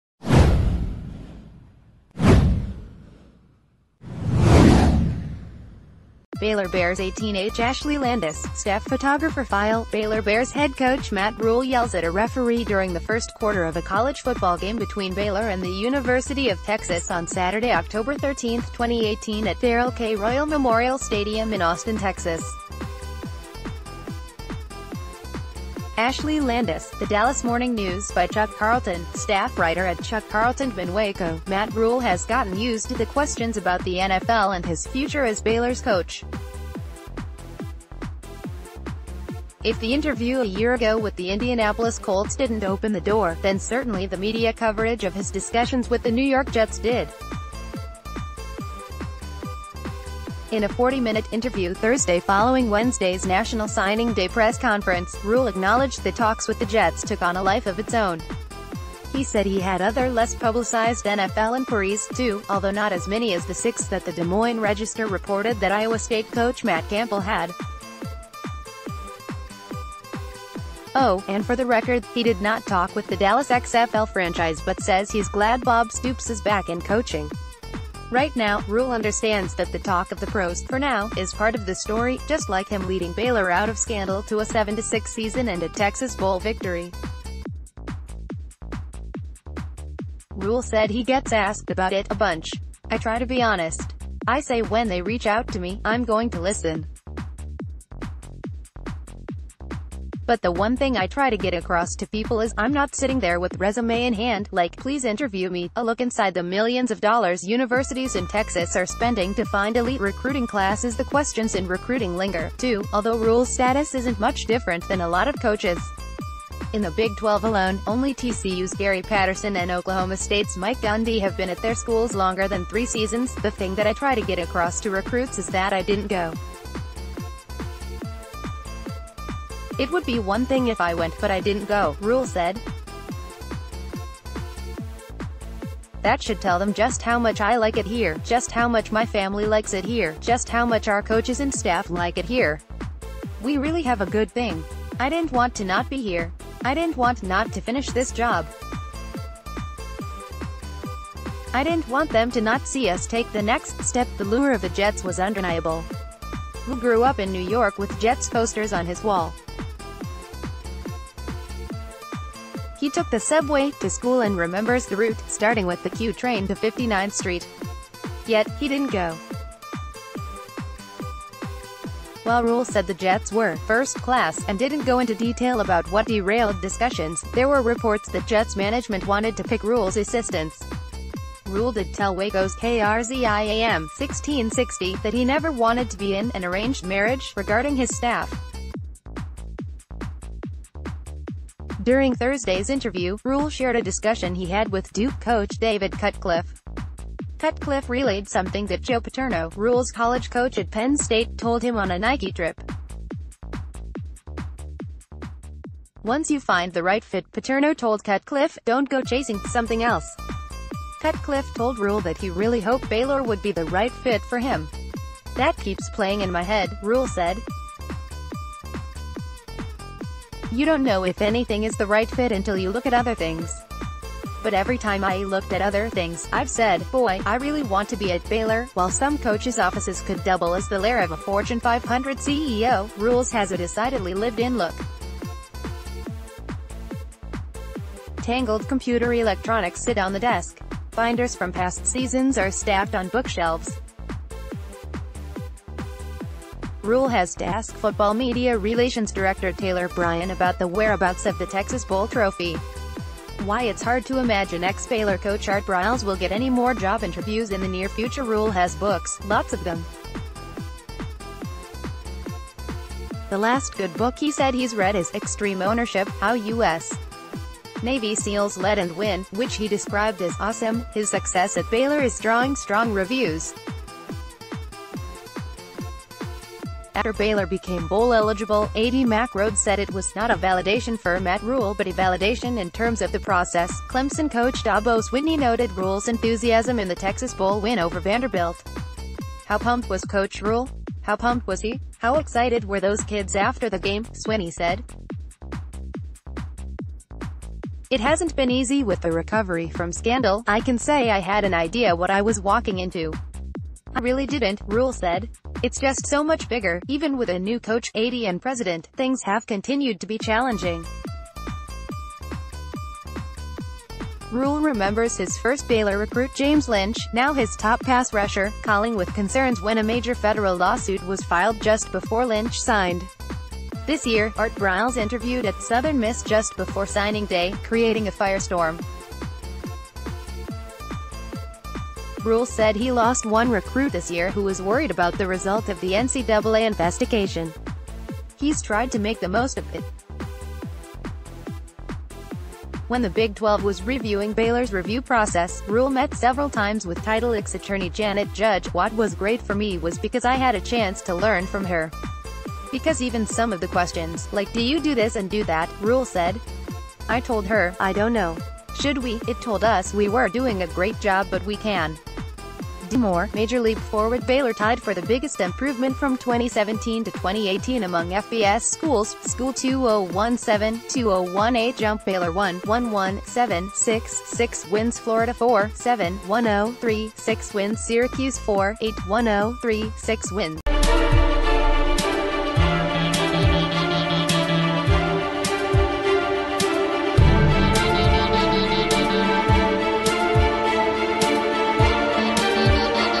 Baylor's Matt Rhule opens up about NFL interviews, recruiting and the piece of advice 'that keeps...
Baylor's Matt Rhule opens up about NFL interviews, recruiting and the piece of advice 'that keeps playing in my head'

Baylor Bears 18h Ashley Landis/Staff Photographer FILE - Baylor Bears head coach Matt Rhule yells at a referee during the first quarter of a college football game between Baylor and the University of Texas on Saturday, October 13, 2018 at Darrell K Royal Memorial Stadium in Austin, Texas. (Ashley Landis/The Dallas Morning News) By Chuck Carlton , Staff Writer @ChuckCarltonDMN WACO -- Matt Rhule has gotten used to the questions about the NFL and his future as Baylor's coach. If the interview a ye...